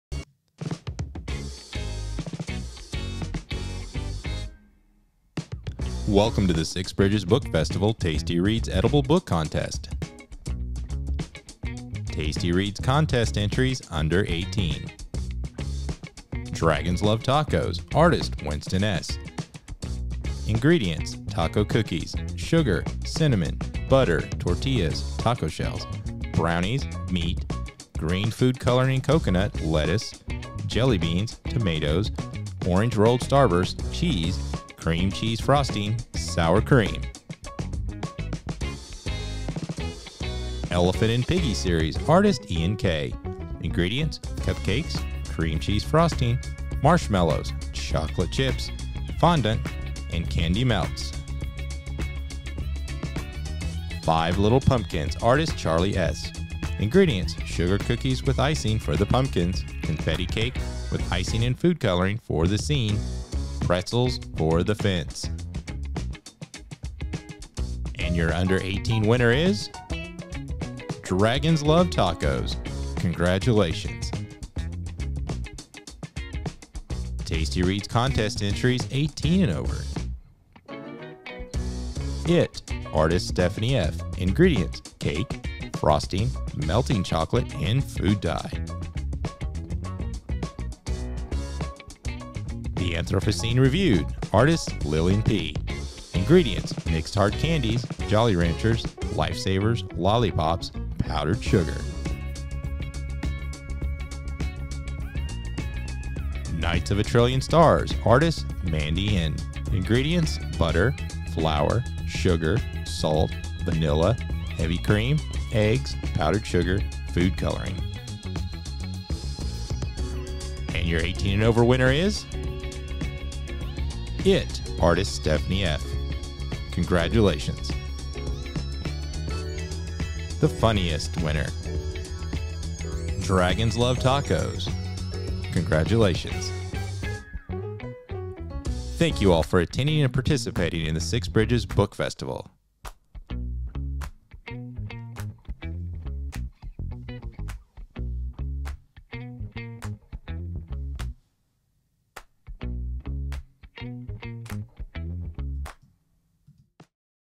Tasty Reads
Tasty Reads, CALS’ Edible Book event, is back. We called for book-related entries constructed of EDIBLE substances. The public was asked to vote in each contest between October 25 and October 30.  Three prizes were awarded: 1) $300 to the best entry in the 18 and Over category, 2) $100 to the best entry in the Under 18 category, and 3) $100 to the entry in either category deemed "funniest". The winning entry in each contest will be announced on Sunday, October 31, at 6:30pm.